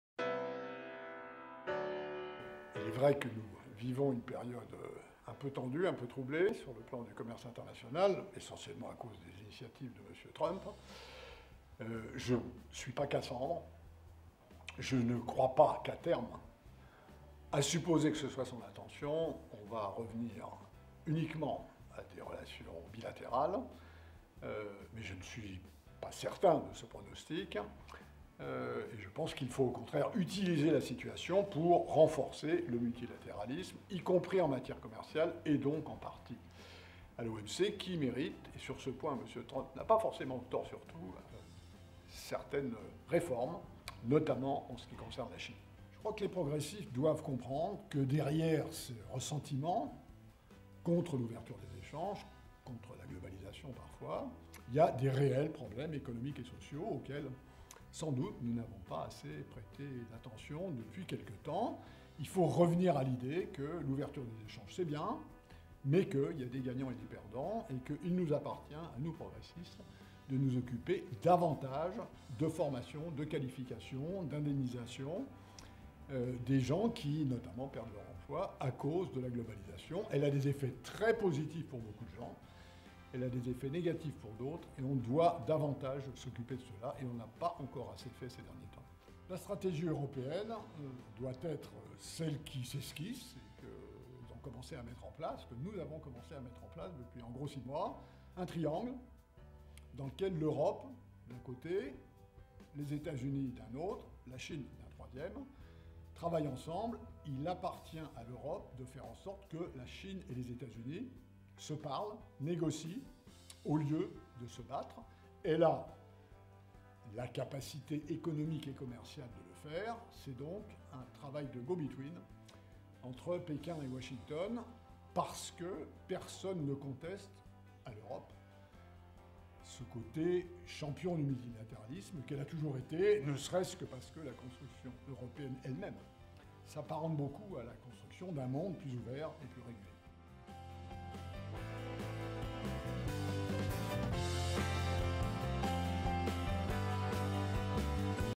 Interview with Pascal Lamy on International Trade and Multilateralism
Interview with Pascal Lamy, President Emeritus at Jacques Delors Institute on International Trade.